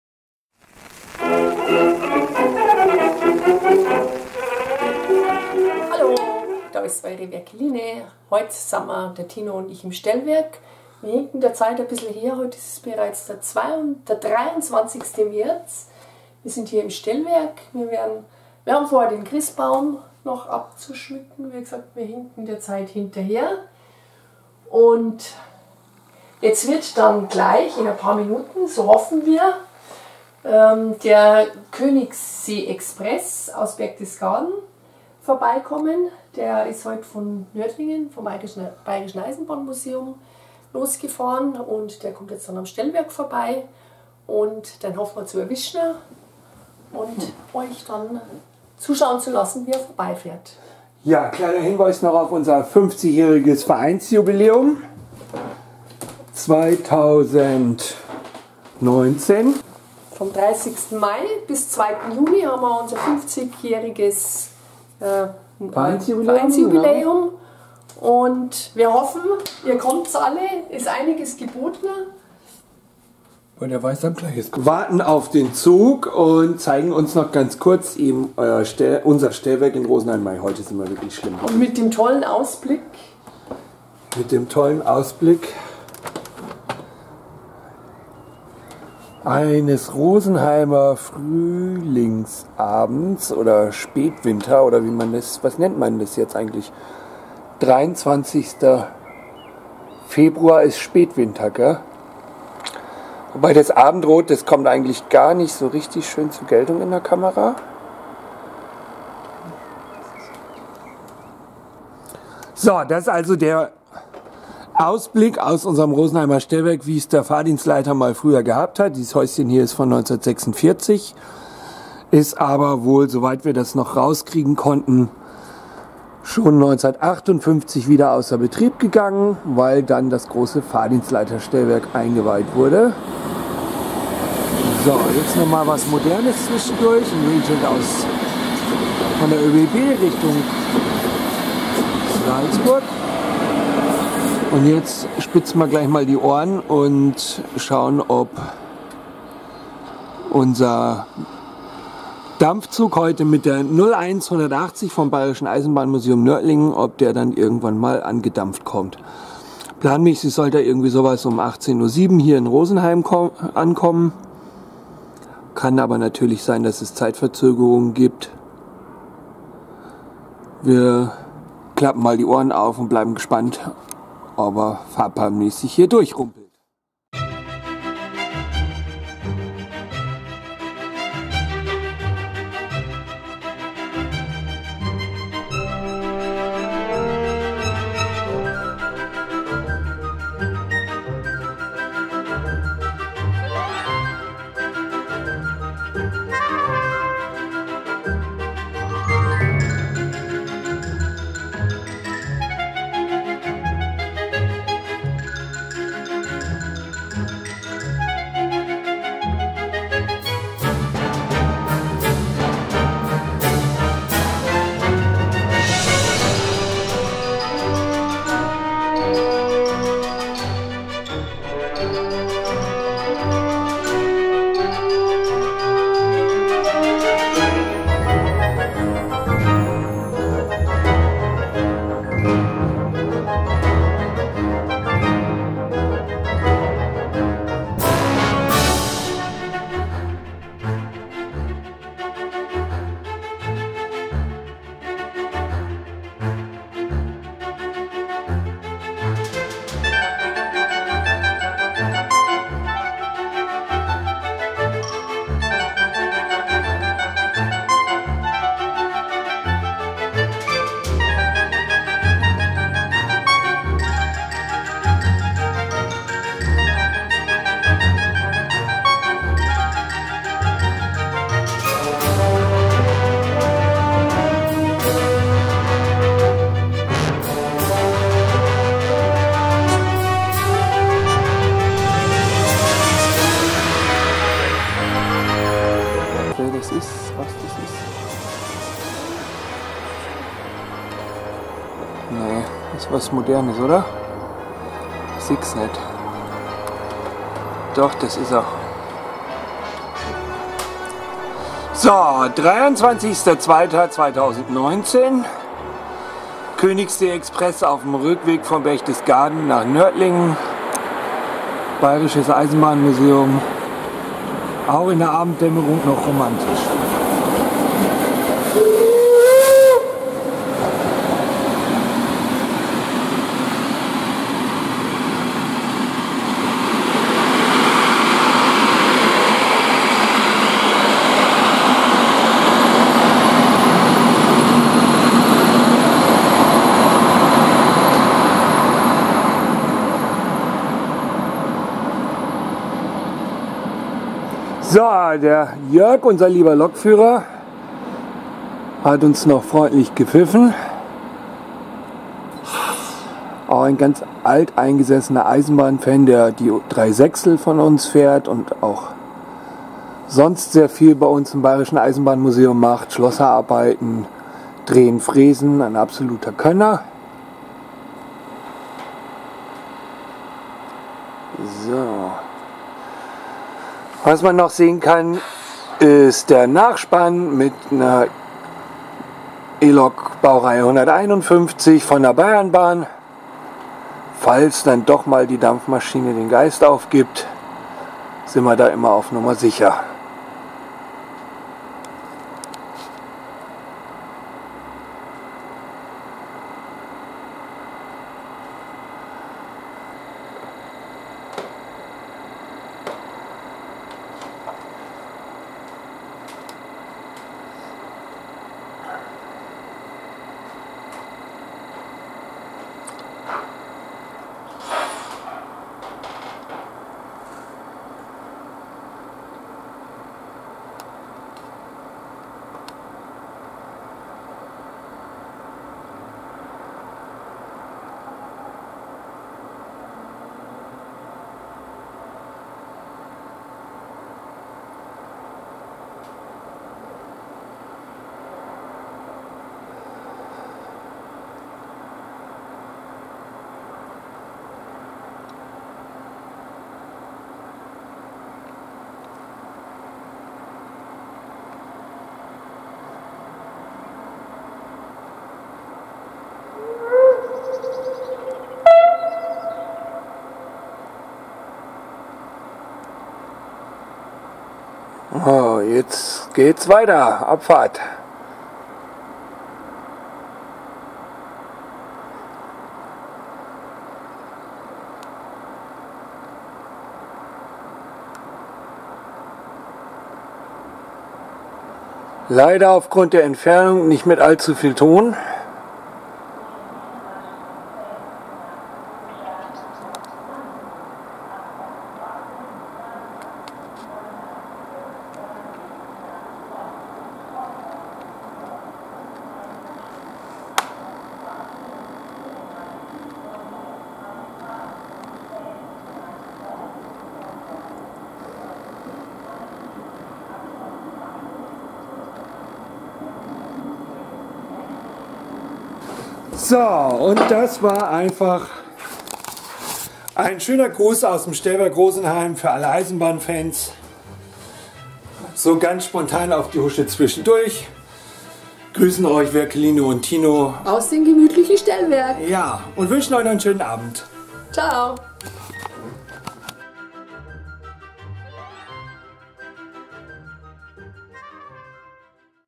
Stellwerk Rosenheim - Vorbeifahrt der 01 180 vom Bayerischen Eisenbahnmuseum Nördlingen
Servus zusammen !
Wir haben uns heute am 23.2. bei herrlichstem Sonnenschein dazu entschlossen, in unser gemütliches Stellwerk nach Rosenheim zu fahren. Da Werkeline leider erkältet ist, können wir nicht mit dem Königssee-Express vom Bayerischen Eisenbahnmuseum mitfahren.
Der Dampfsonderzug fährt heute von Nördlingen über Donauwörth, Augsburg, München vorbei an unserem Stellwerk in Rosenheim weiter nach Freilassing und dann nach Berchtesgaden.

Den Nachmittag haben wir dazu genutzt, die Dieselleitungen an unserem Magirus-Deutz Mercur neu abzudichten und die Diesel-Förderpumpe samt Dieselfilter auszutauschen.

Der historische Dampfzug unserer Museumseisenbahn ist mit der Schnellzuglok 01 180 bespannt und fährt gegen 18:00 Uhr am Stellwerk Rosenheim vorbei. Nachspann mit der BR 151 der Bayernbahn GmbH.

Klar daß wir uns das nicht entgehen lassen und trotz Husten und Schniefnase einen kurzen Videoclip vom Sonderzug aufnehmen und hier für Euch einstellen, denn in diesem Frühjahr feiert das Bayerische Eisenbahnmuseum das 50-jährige Bestehen und wir möchten als aktive Mitglieder des Eisenbahnmuseums kräftig Werbung machen, damit unsere Jubiläumsveranstaltung jede Menge eisenbahnbegeisterte Besucher bekommt.

Viel Spaß mit Werkelines kleinem Clip aus unserem Stellwerk Rosenheim !